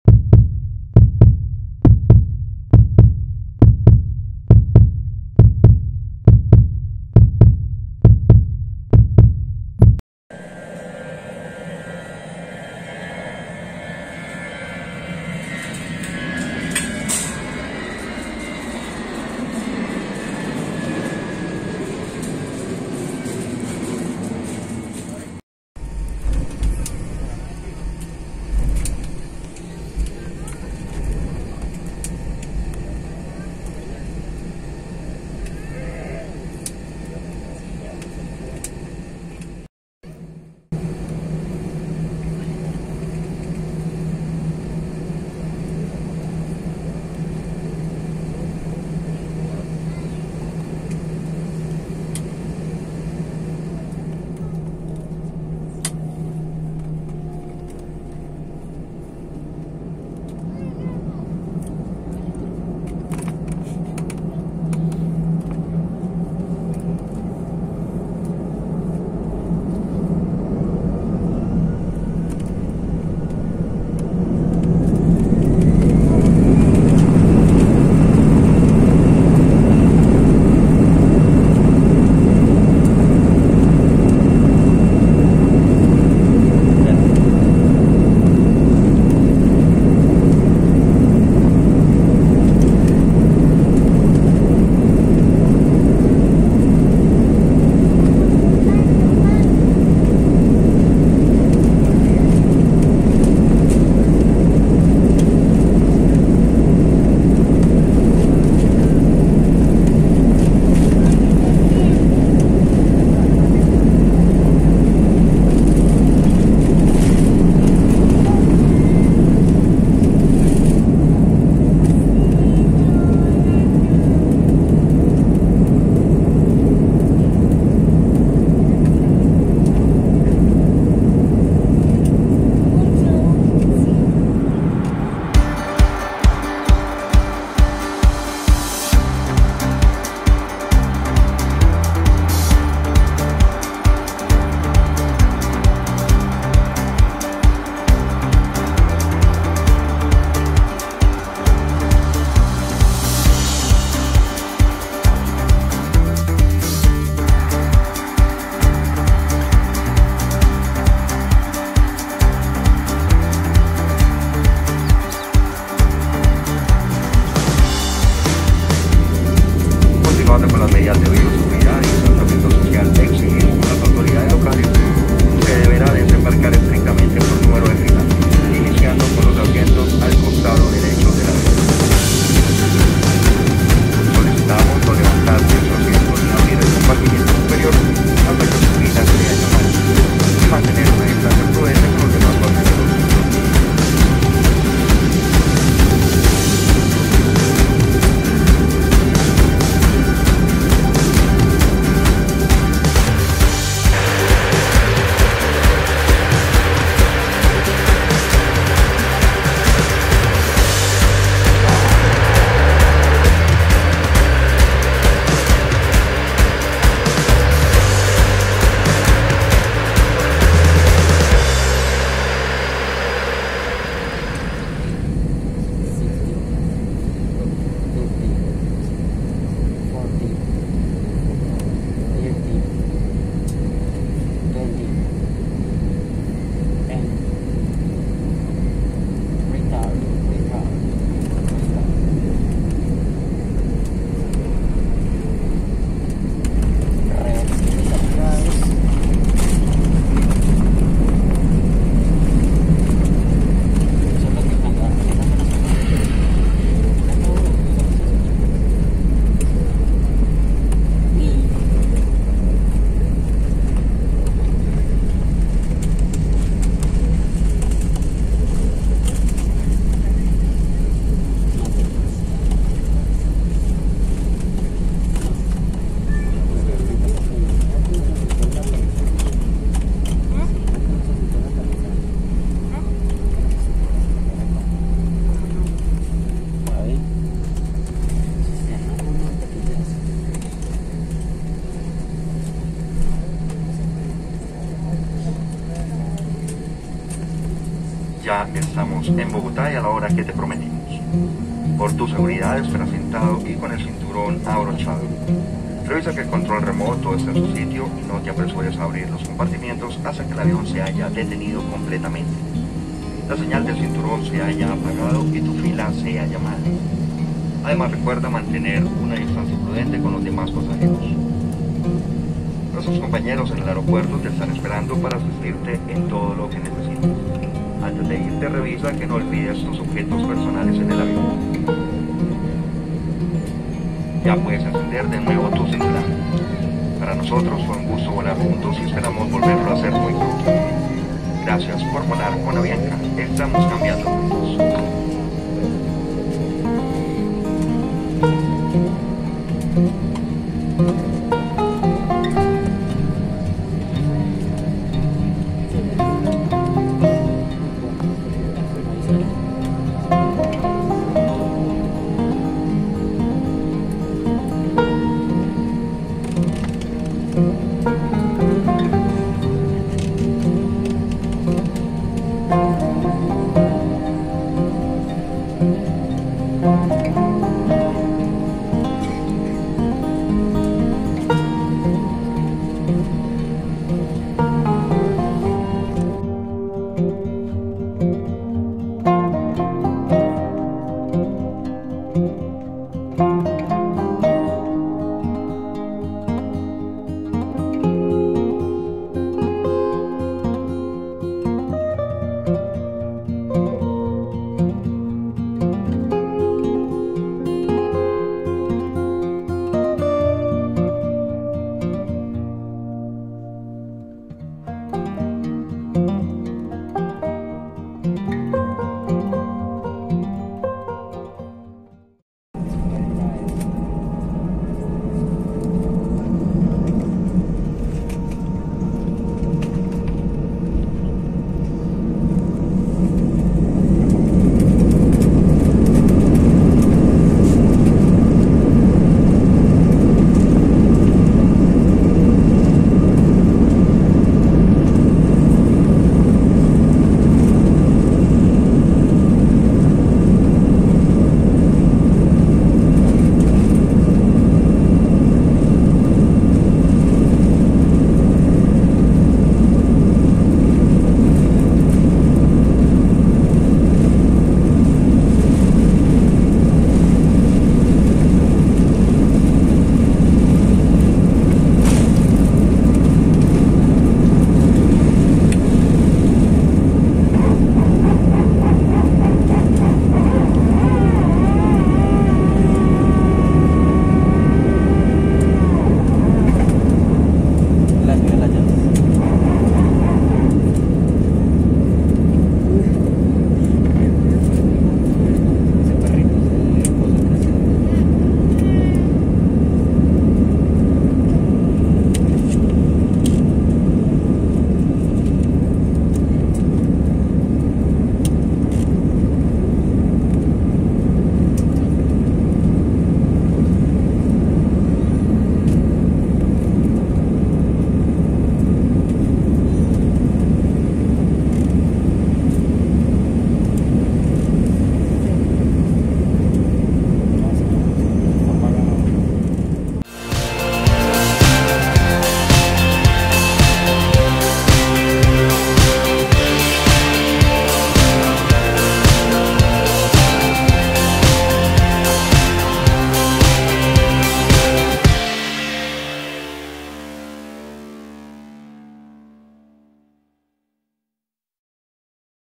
|TRIP REPORT| Avianca A320 | Medellín - Bogotá | Mismo Día | Espectacular Aproximación HD
Completo reporte de vuelo con Avianca en Airbus A320 en la ruta Medellín-Bogotá-Medellín y una excelente aproximación, además despegue nocturno.

Activa las notificaciones y campana de YouTube para que te avisen al instante con cada nuevo vídeo nuestro.